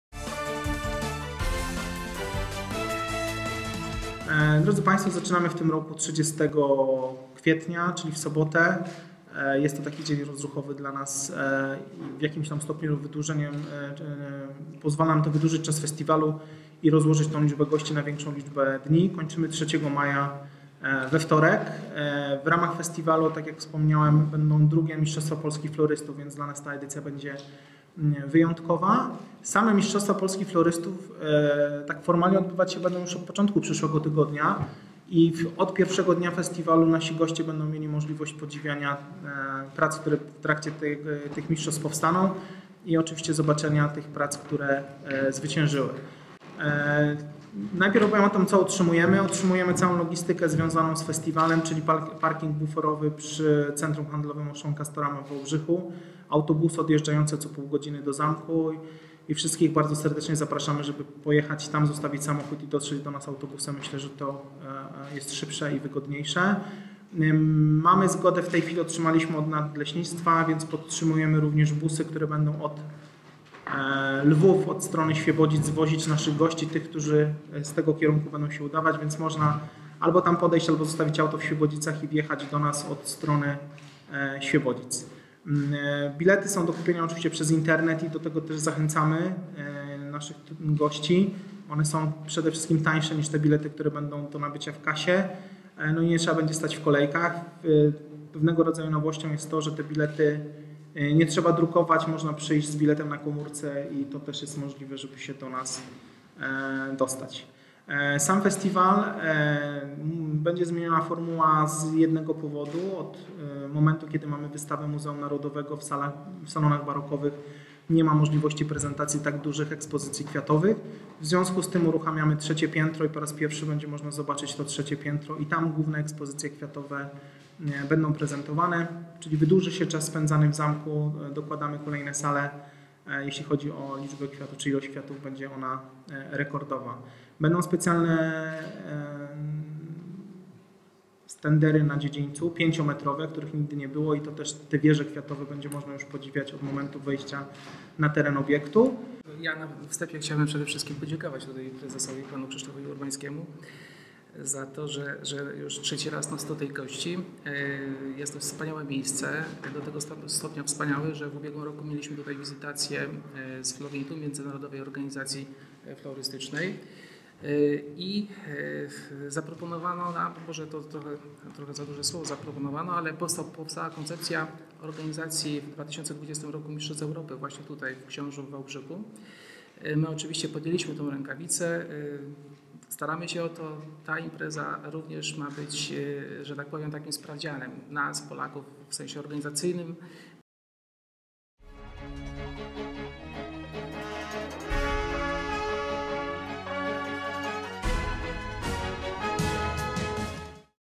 Kwiaty opanują Zamek Książ
Bitwa florystyczna, 5-metrowe konstrukcje kwiatowe, 250 wystawców, II Mistrzostwa Polski Florystów oraz szereg innych atrakcji przygotowali organizatorzy XXVIII Festiwalu Kwiatów i Sztuki, który odbędzie się na Zamku Książ w dniach 30 kwietnia – 3 maja. W tym roku po raz pierwszy goście będą mogli zwiedzić zmodernizowane III piętro Zamku.